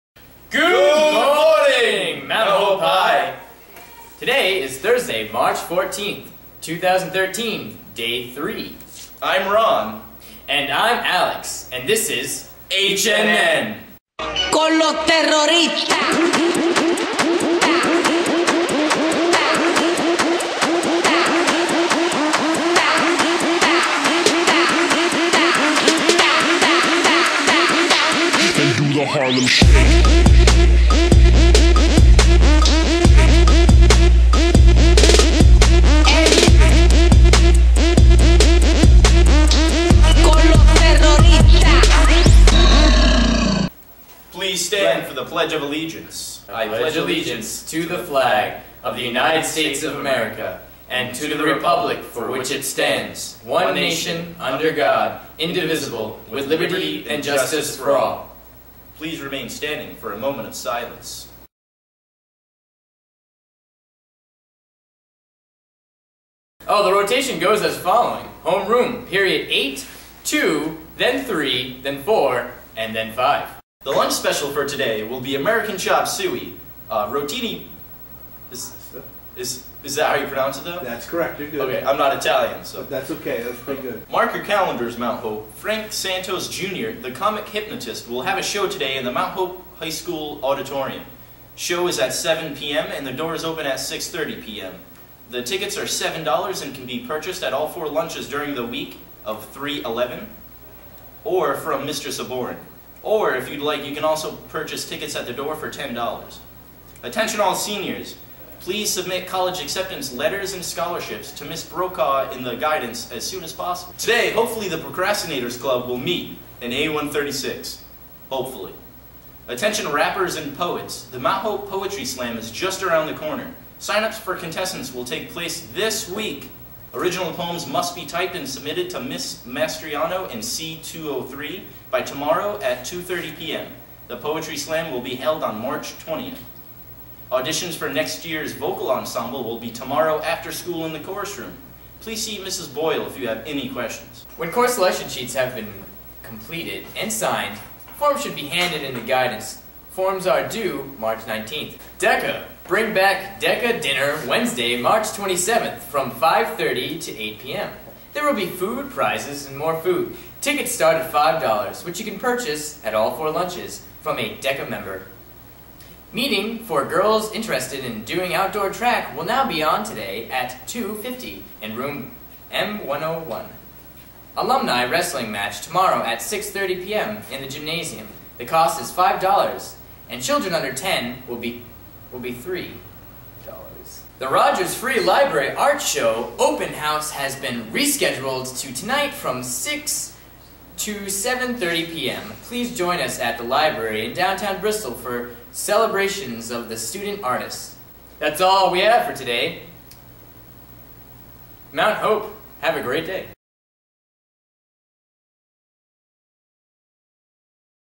HNN March 14th
mt hope husky news network with former students Alex Watrous and Ron Sadler.